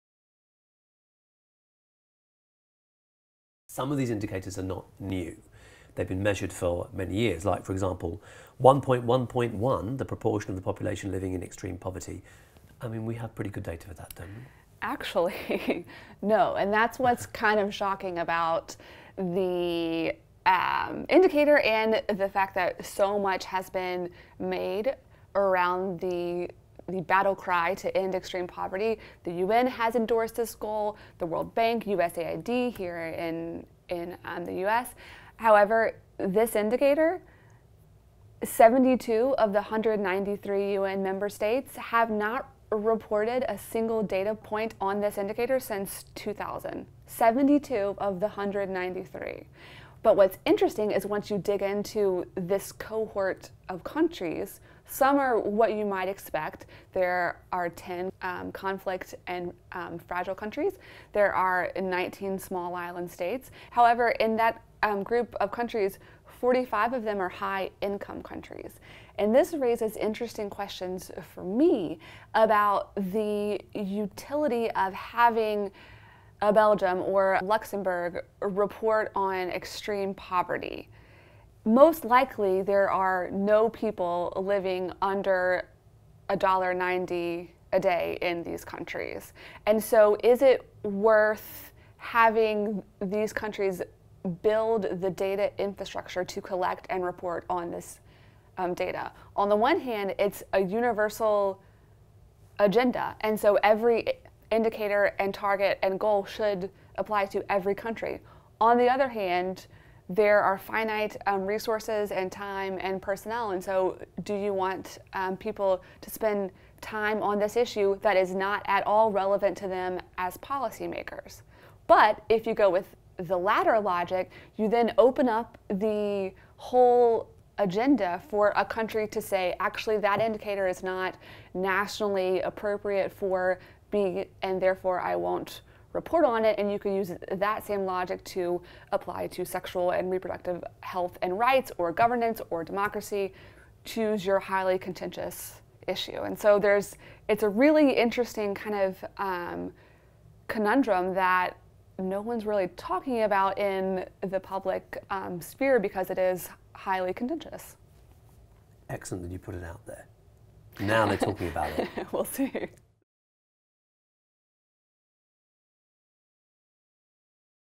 How Will We Measure the SDGs? – Casey Dunning | CGD Podcast
If the SDGs answered the “what” question of the 2030 development agenda, the 230 newly approved indicators answer the “how.” But as CGD policy analyst Casey Dunning tells me on this week’s podcast, those 230 indicators raise new questions as well.